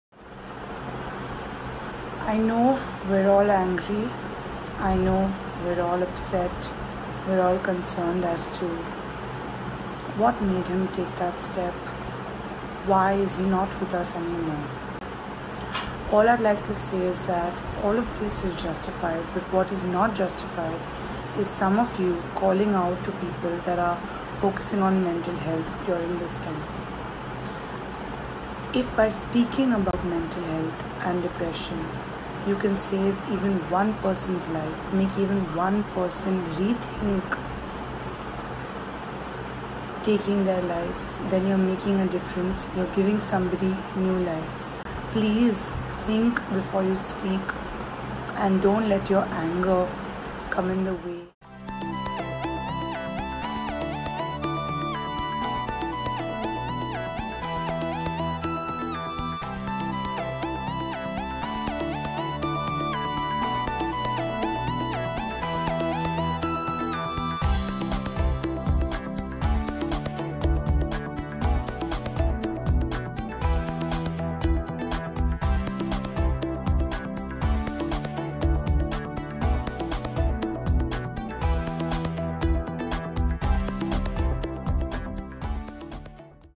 Benafsha Soonawalla Urges Everyone To Speak About Mental Health
Benfasha Soonwalla encourages everyone to open up about their mental health, to influence each other as much as they can to heal themselves and not take their life.